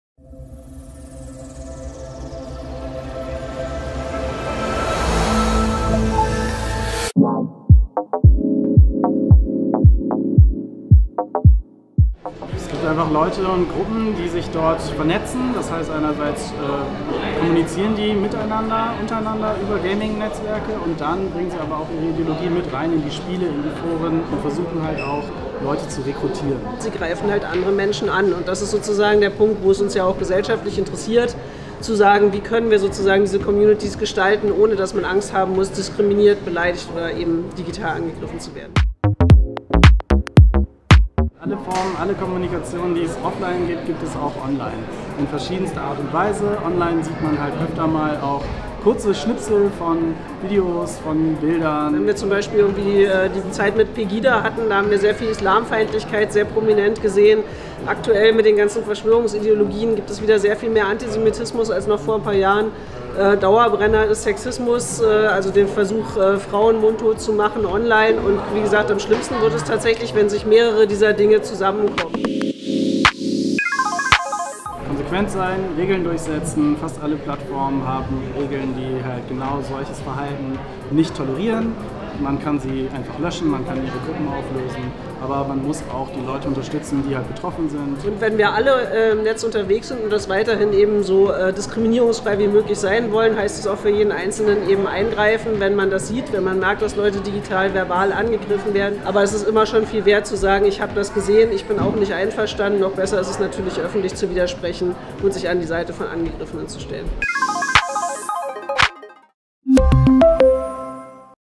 3 Fragen an die Amadeu Antonio Stiftung - Magenta Moon
Online sowie vor Ort gibt es ein vielfältiges Programm mit Talks und Workshops zu Themen wie Zivilcourage, Medienkompetenz, Optimismus und digitaler Teilhabe. Seid